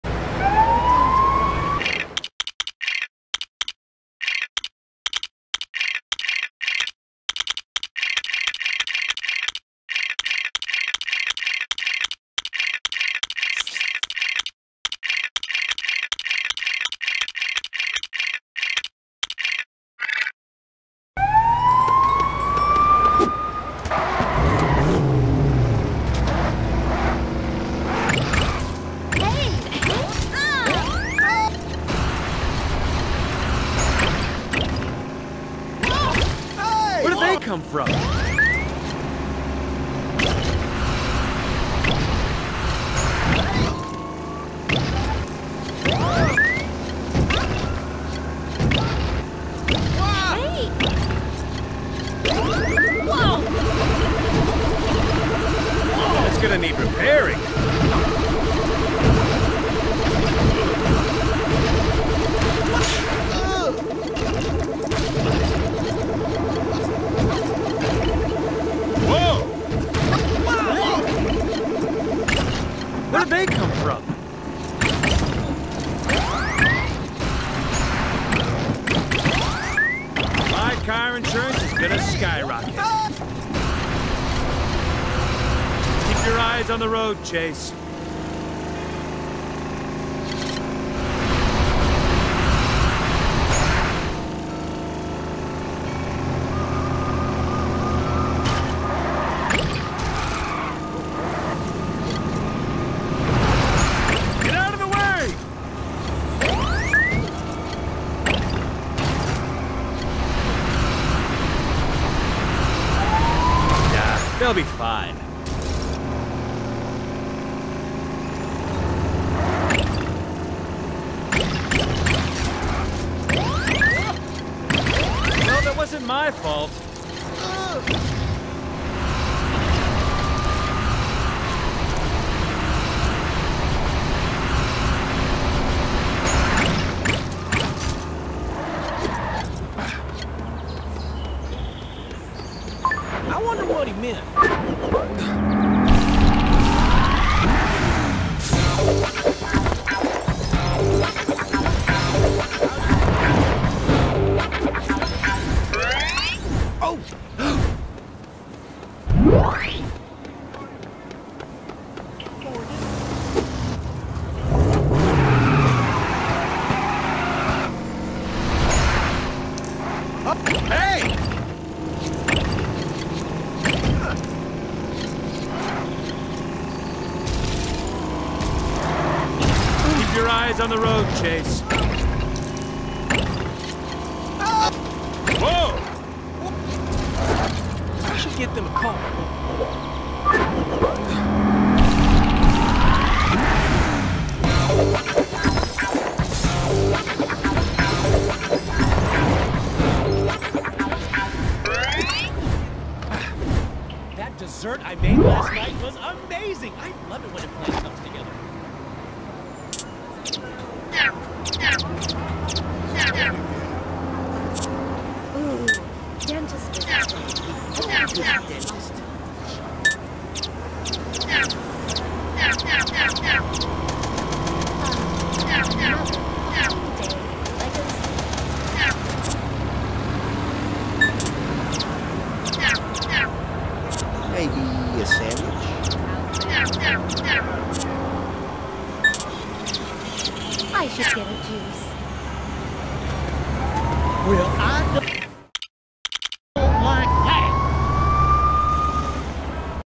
Super Builds in Fresco I 100% Items Collection (Spoiler Alert)
Fresco super builds

I will not state any references to famous movies. You can however post the references you see in the comments thread.